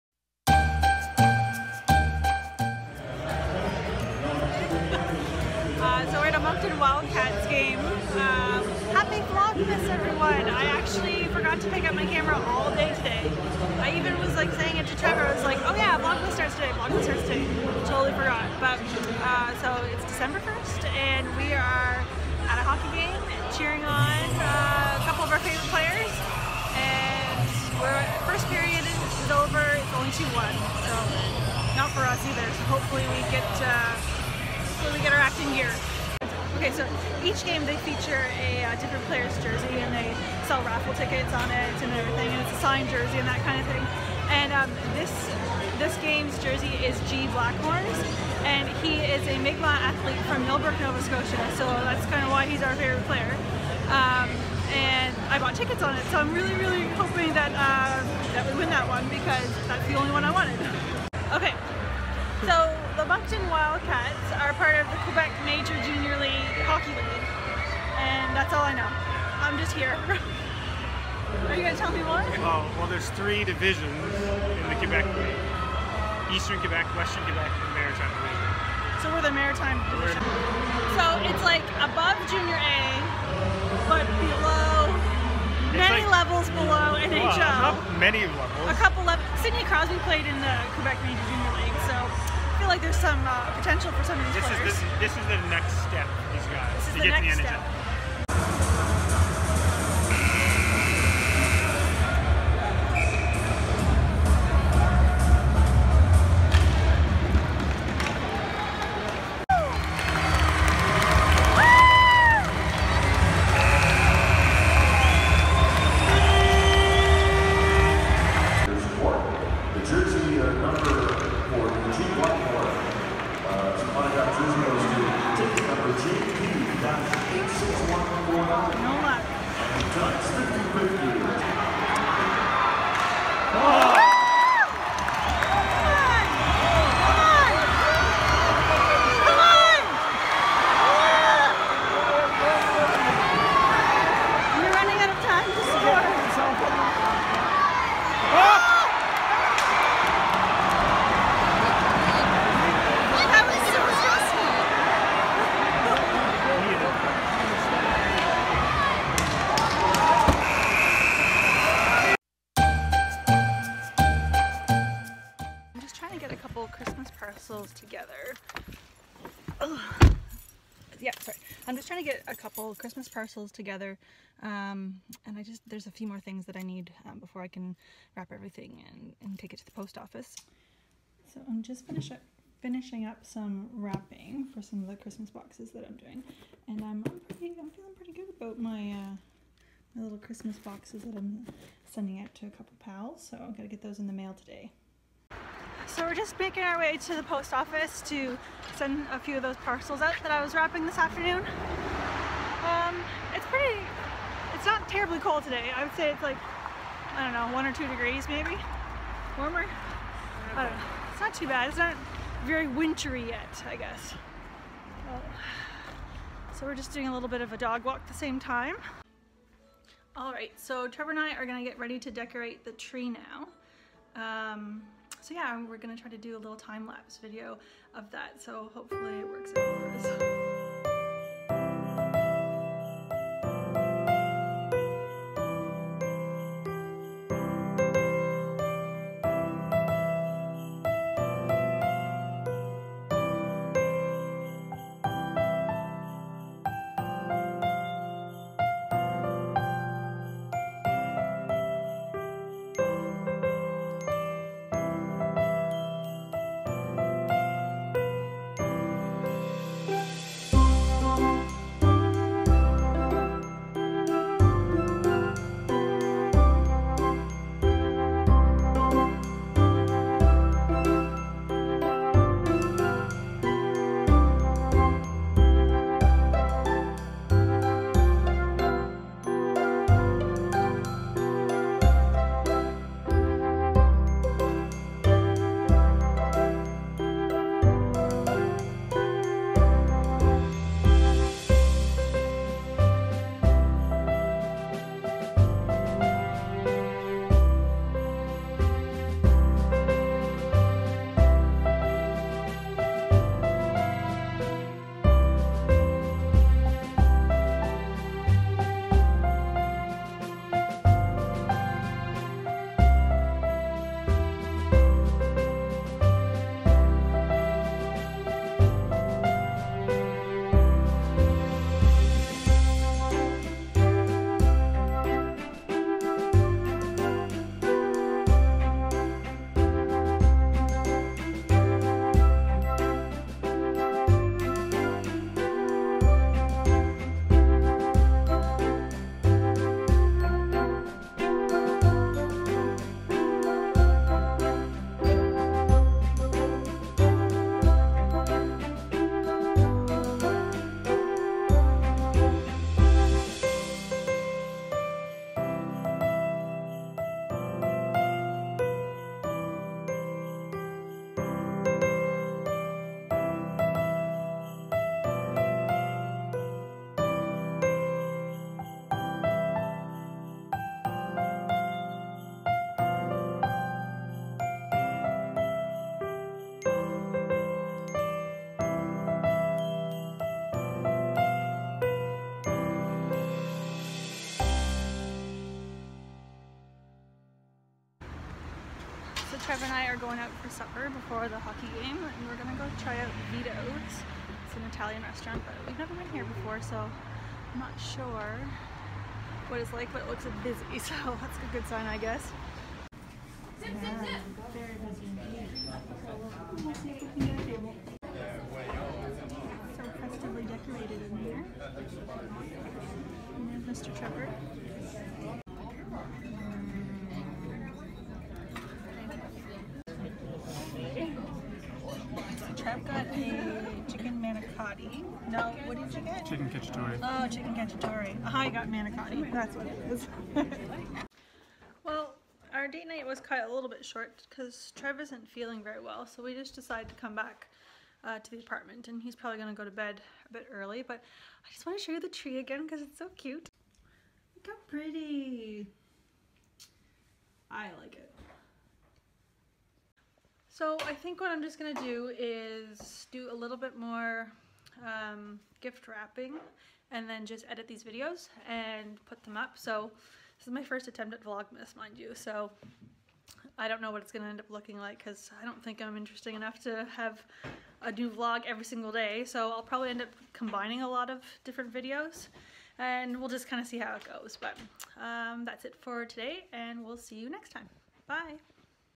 HOCKEY GAME & CHRISTMAS TREE DECORATING | Vlogmas Day 1 & 2
Cheering on Moncton Wildcats and our fav player - G. Blackmore! Sending some Christmas parcels and decorating the tree!! :)

Happy Vlogmas Days 1 & 2